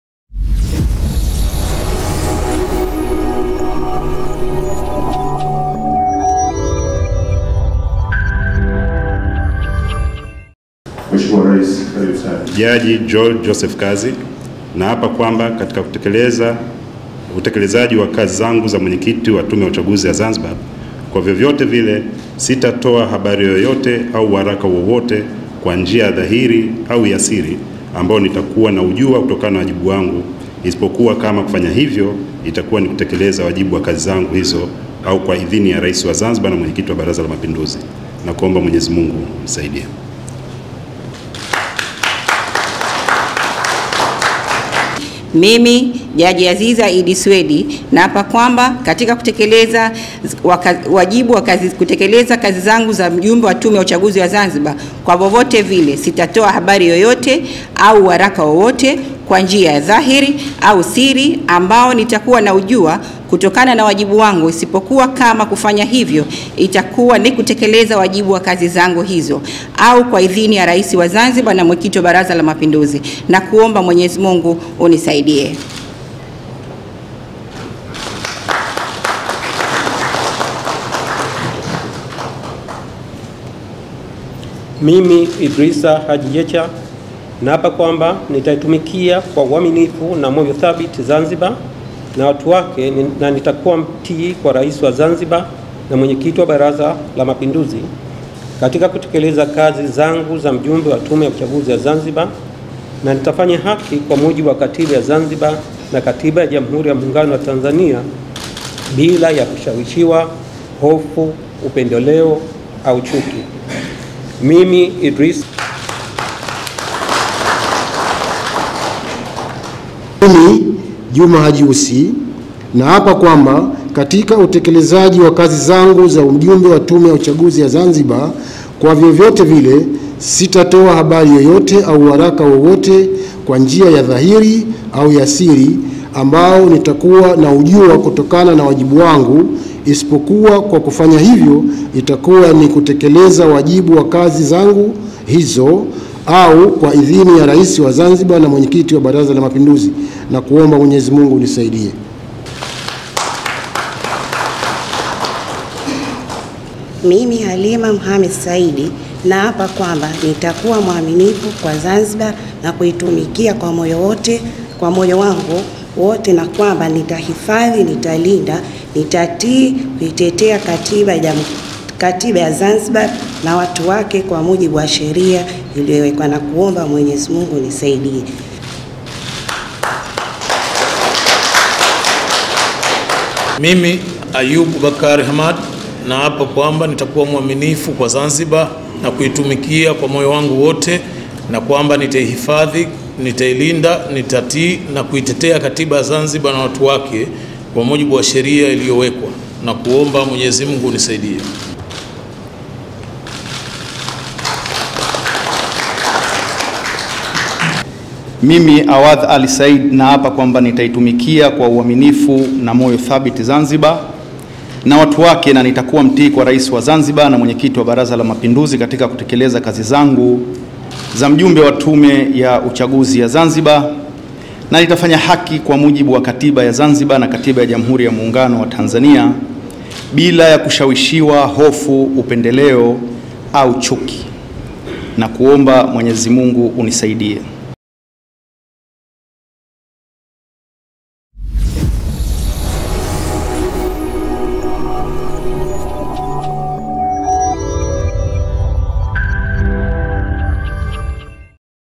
Dk. Mwinyi amewaapisha viongozi wapya wa Tume yaUchaguzi ya Zanzibar ZEC
Zanzibar,
28 Agosti, 2023
TAARIFA KWA VYOMBO VYA HABARI
RAIS wa Zanzibar na Mwenyekiti wa Baraza la
Mapinduzi Dk. Hussein Ali Mwinyi,
amewaapisha viongozi wapya wa Tume ya
Uchaguzi ya Zanzibar (ZEC) akiwemo
Mwenyekiti wa tume hiyo, Jaji George Joseph
Kazi na wajumbe sita wa tume hiyo. Tukio hilo
lilifanyika Ikulu Zanzibar.

Wajumbe walioapishwa na Rais Dk. Mwinyi ni
pamoja na Jaji Aziza Iddi Suwedi, Idrisa Haji
Jecha, Juma Haji Ussi, Halima Mohamed Said,
Ayoub Bakari Hamad na Awadh Ali Said.

Viongozi hao wa ZEC waliteuliwa na Rais Dk.
Mwinyi, Agosti 14 mwaka huu.